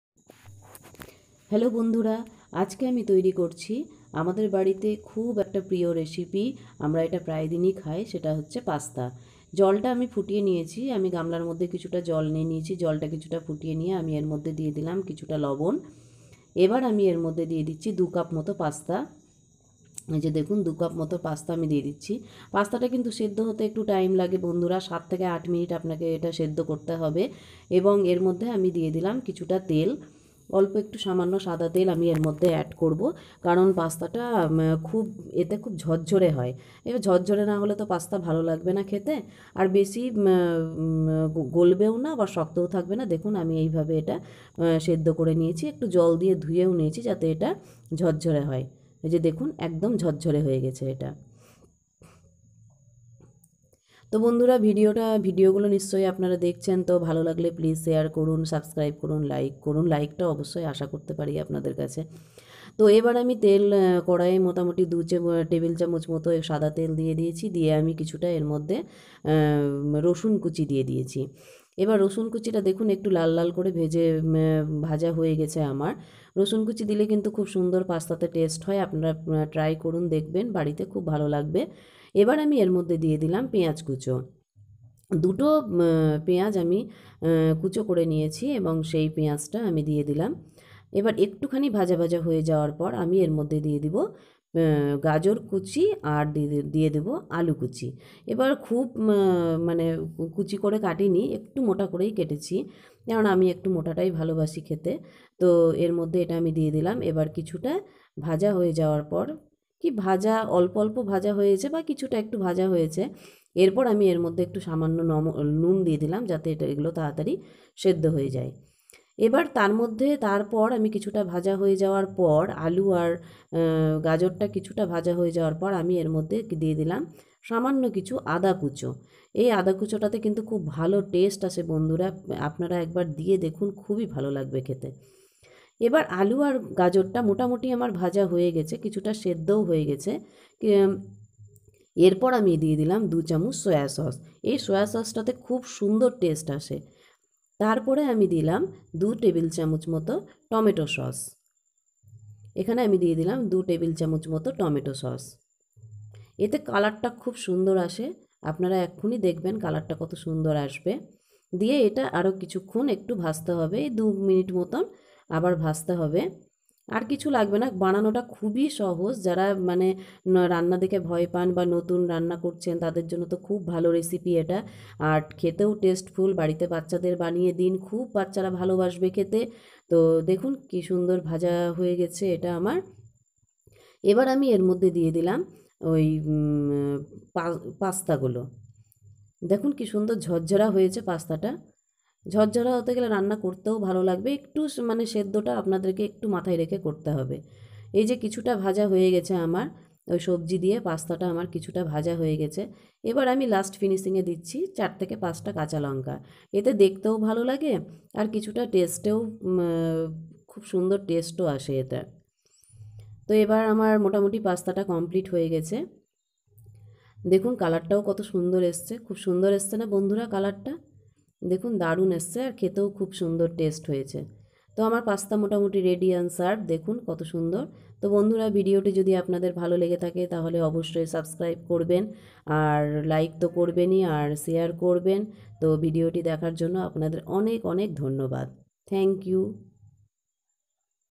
salpo upokoron diyei mukhorochok pasta ready.
quick and easy pasta recipes with few, ingredients vegetarian